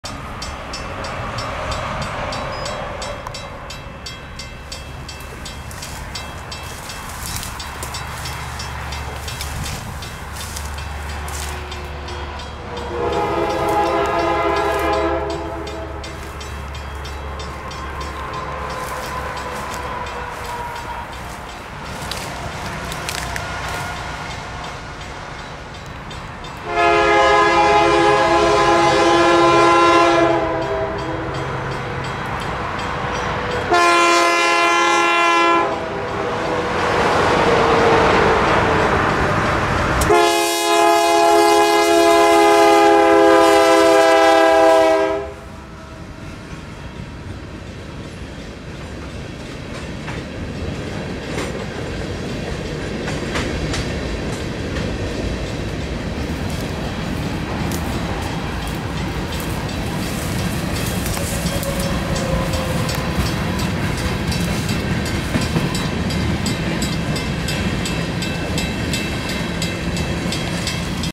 Canadian Pacific No.9532 GE AC44CW Running Backwards Over HWY 321 In Lincolnton NC 8-24-21
Not edited in any way, uploaded straight from the SD card with the wandering through the tall grass included. I was just leaving my day job when I heard a train on the CSX tracks coming from Charlotte. I rushed over to Gaston street to get a shot of the train crossing the highway only to realize the grass was as tall as I am! It was my first time seeing an engine this big running long hood forward at more than a crawl.